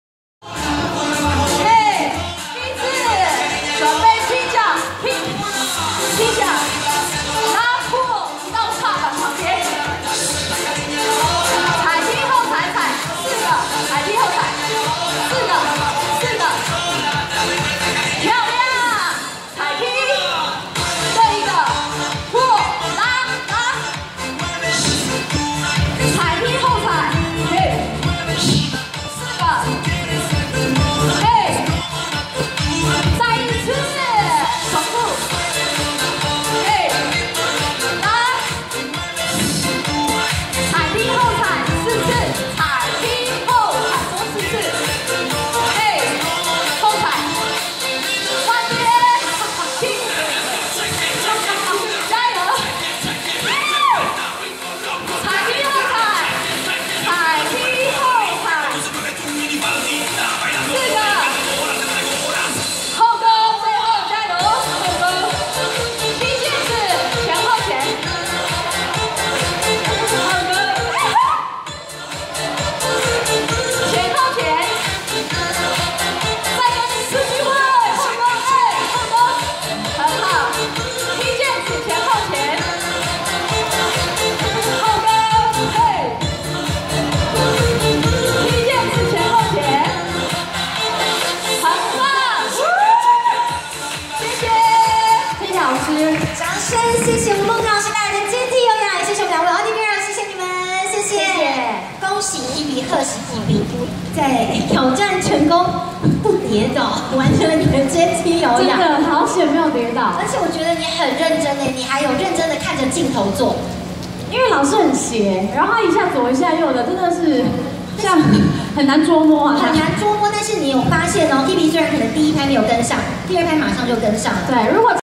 Hebe整慘自己直播玩有氧 大喊還好沒跌倒
剛結束高雄演唱會的Hebe田馥甄，今天馬不停蹄為知名運動品牌站台，在現場率領adigirls一起挑戰階梯有氧等運動訓練，並首度在社群網站上直播，和粉絲零時差互動。